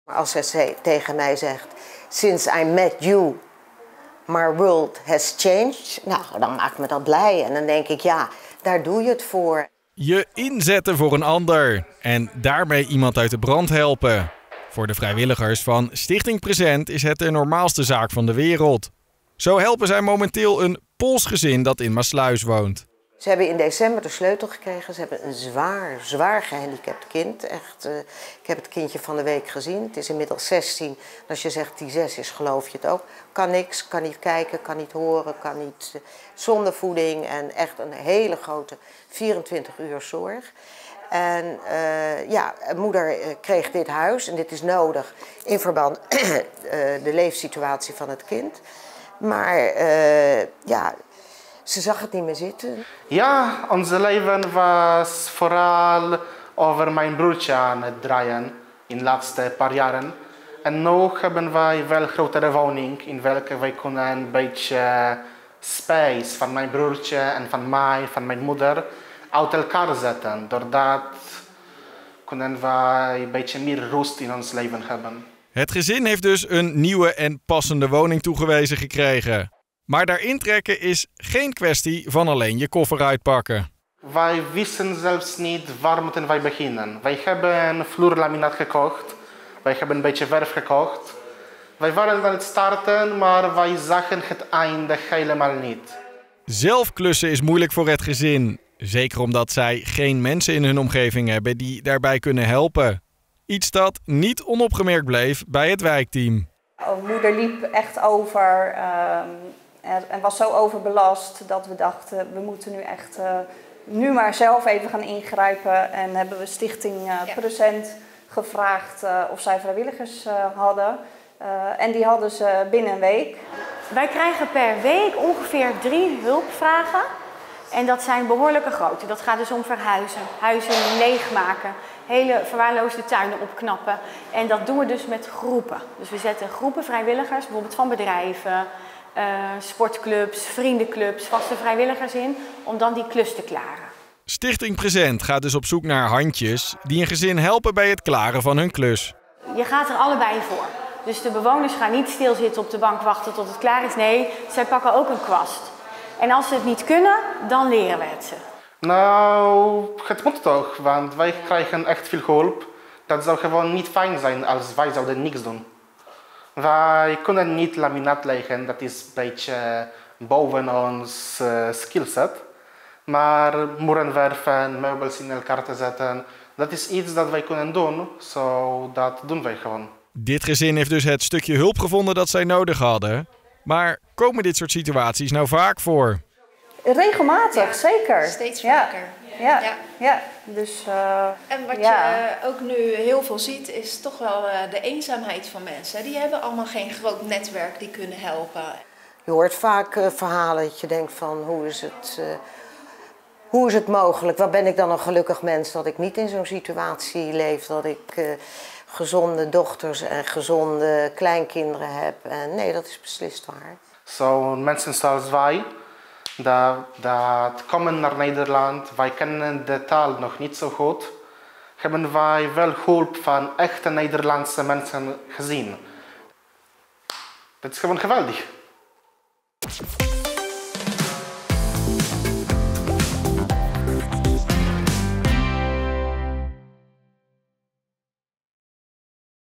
Stichting Present: 'Dankzij jou is mijn wereld mooier'
Opruimen, schoonmaken, schilderen of een luisterend oor. Op die manier helpen vrijwilligers van Present de mensen die dit nodig hebben. Zo ook in Maassluis.